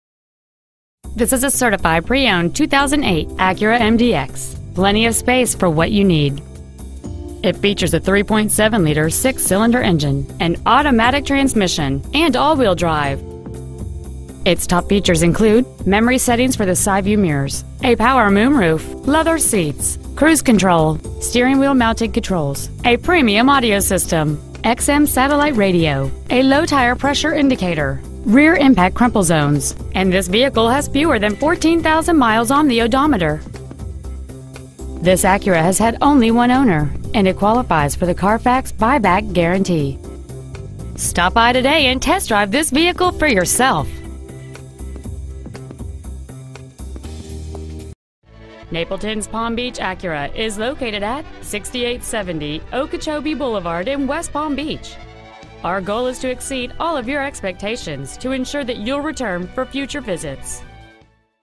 Preowned 2008 Acura MDX Miami FL
Year: 2008
 Make: Acura
 Model: MDX
 Engine: 3.7L PGM-FI SOHC 24-valve VTEC V6 engine
 Trans.: Automatic
 Exterior: Blue
 Miles: 13,955
 Interior: Other

 Preowned 2008 Acura MDX West Palm Beach FL